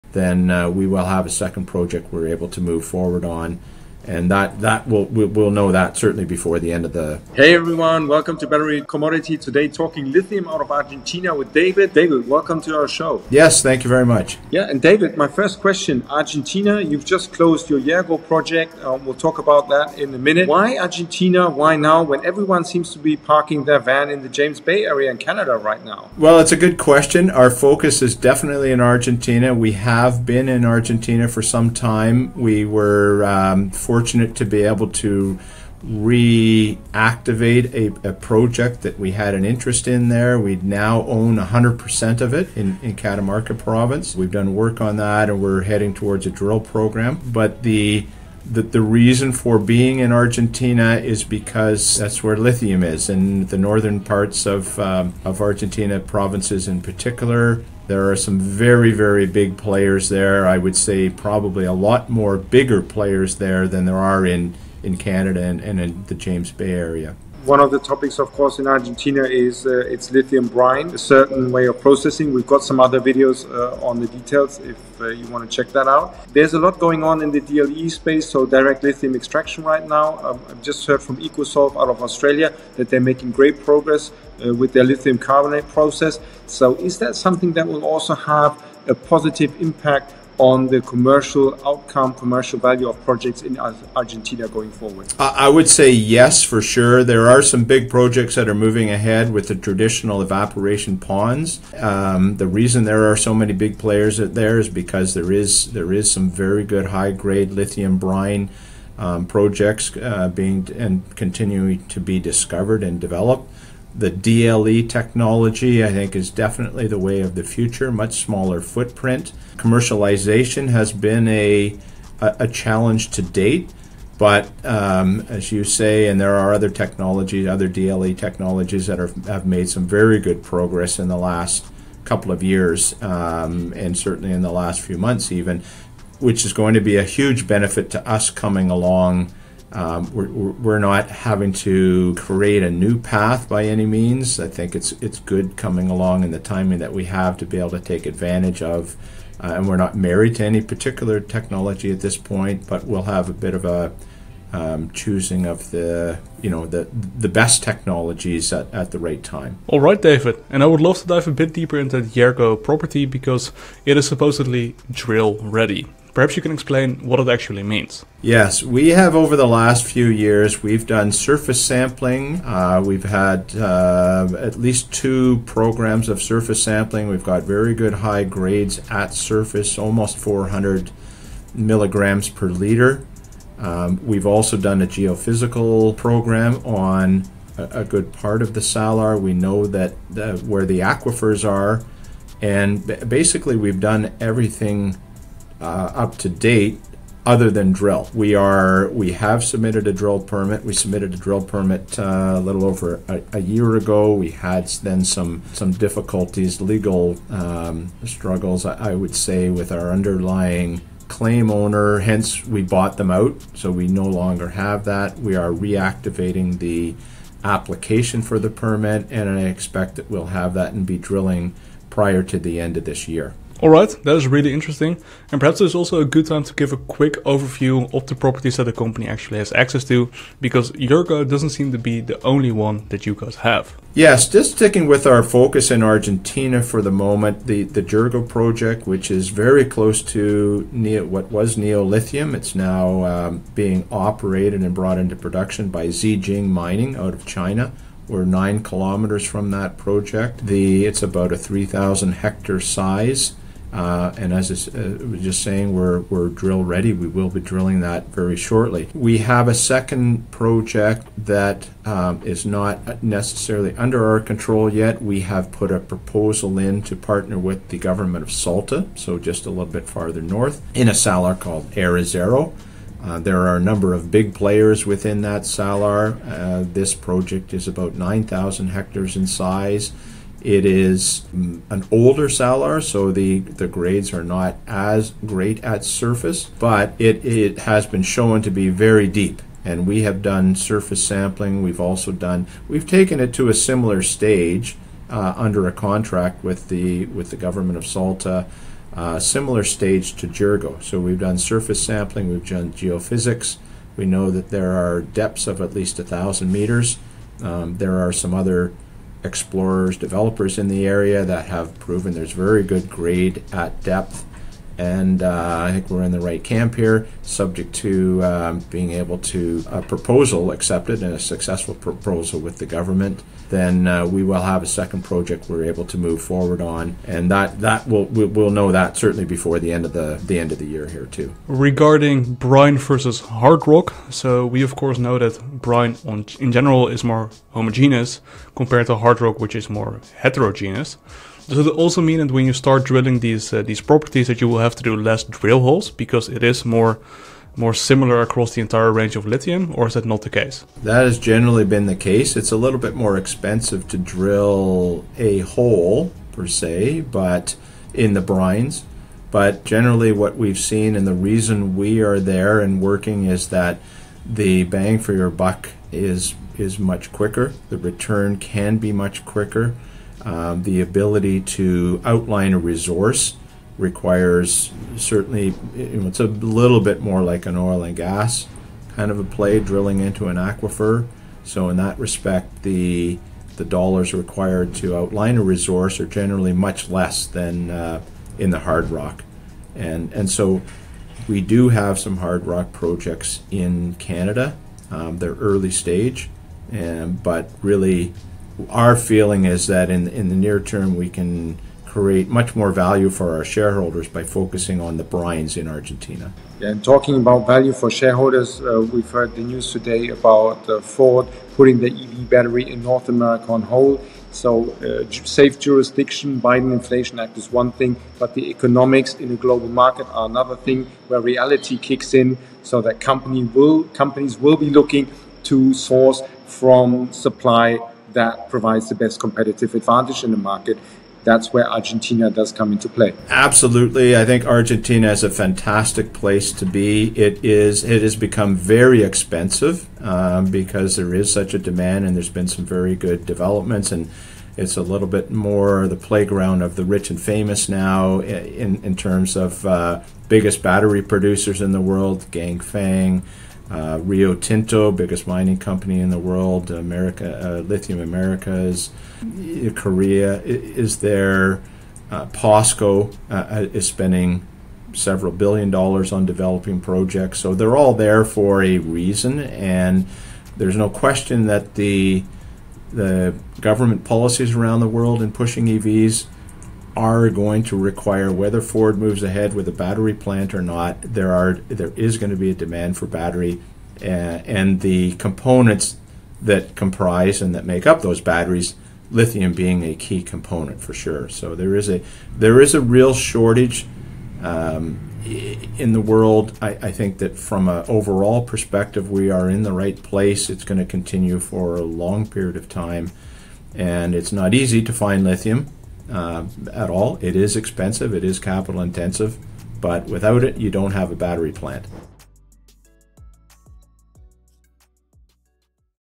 (Almost) Everything You Need to Know about Portofino | $POR Stock
Talking with the CEO of Portofino about the company, how it's currently structured, what properties the company has & what the catalysts an investor at home can expect are!

00:00 – Intro
00:14 – Most Recent Acquisition!
01:07 – DLE: How Direct Lithium Extraction will make a Difference!
02:40 – “Drill Ready” Explained
03:43 – Overview of Argentina Properties
05:31 – Hard Rock vs Brine Lithium Drilling
06:46 – Ford Difficulties in US Battery Markets

Portofino Resources Inc. | Exploring Lithium Assets in Argentina
Portofino Resources Inc. is a Canada-based mining exploration company focused on advancing lithium assets in Argentina, a key region for global lithium supply.

Stock Listings:
📈 TSXV: POR | OTCQB: PFFOF | FSE: POTA